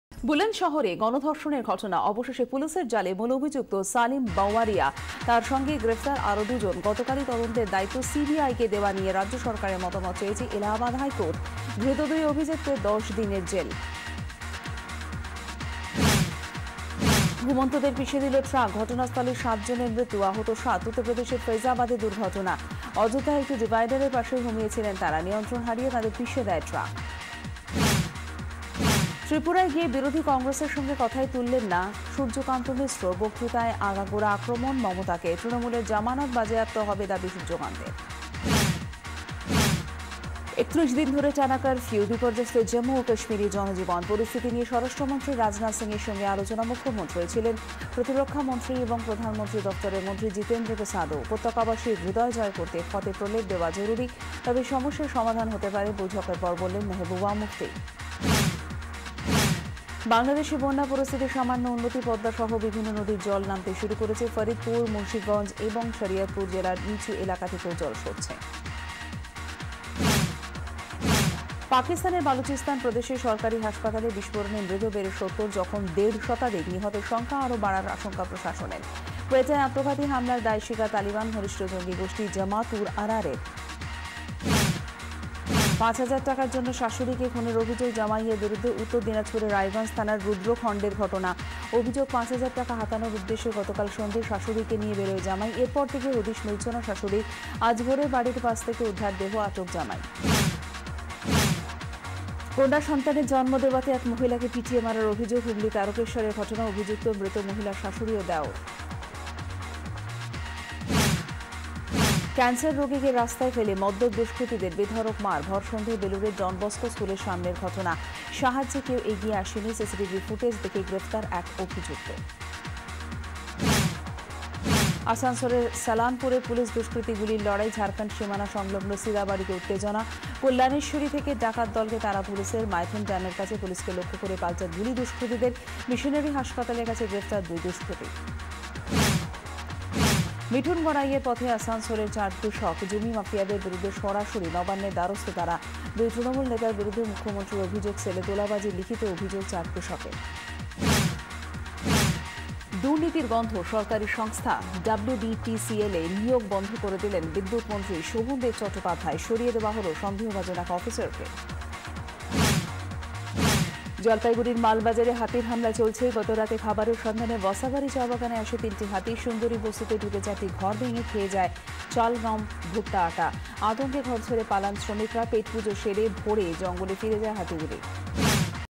Jhatpat 24: Salim Bawaria along with two more arrested
Jhatpat 24: Salim Bawaria along with two more arrested. To satisfy the demands of high-rise favourites of Bengali audiences, Zee 24 Ghanta brings all the latest headlines and news stories.